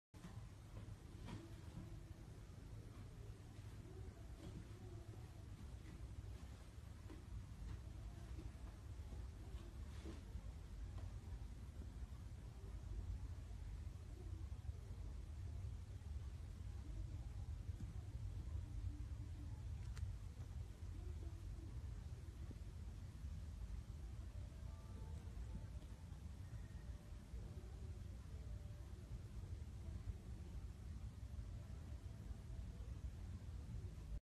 En tatlı uyku
Dünyanın en sevimli uykusuna yatmış kedi kardeşler.